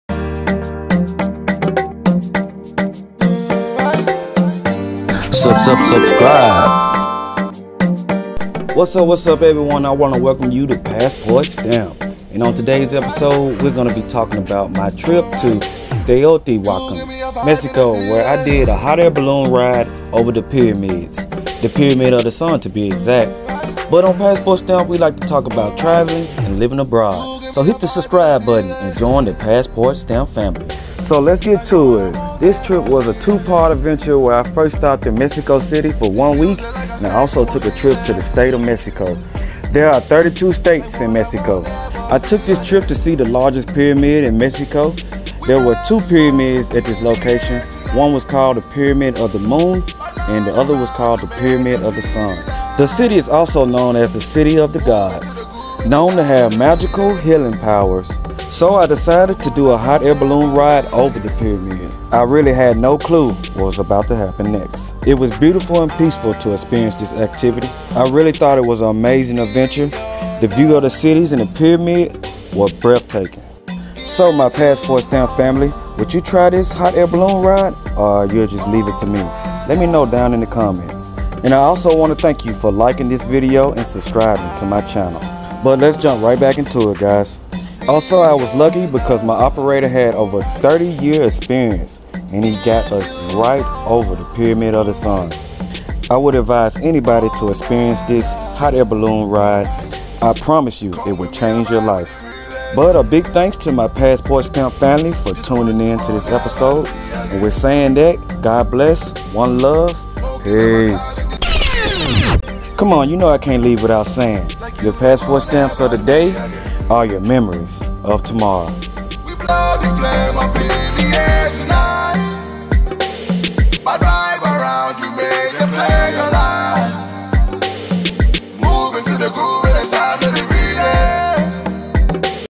MEXICO MAGICAL 🇲🇽
Pyramid of the Sun, large pyramid in the ancient city of Teotihuacán, Mexico, that was built about 100 CE and is one of the largest structures of its type in the Western Hemisphere. I decided to do a hot air balloon ride over the pyramids!